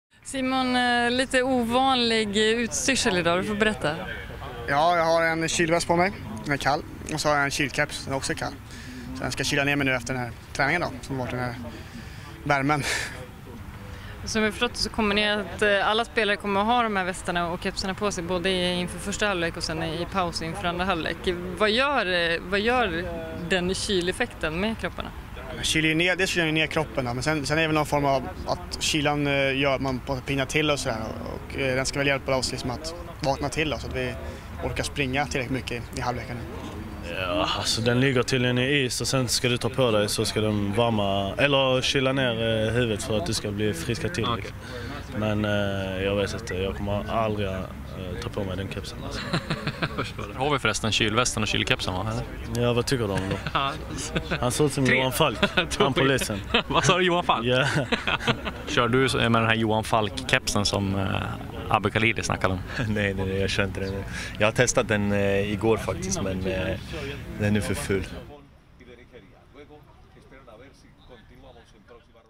Abdulrahman Khalili: "Man ser ju ut som Johan Falk"
Kylkepsen och kylvästen ska hjälpa landslagsstjärnorna mot hettan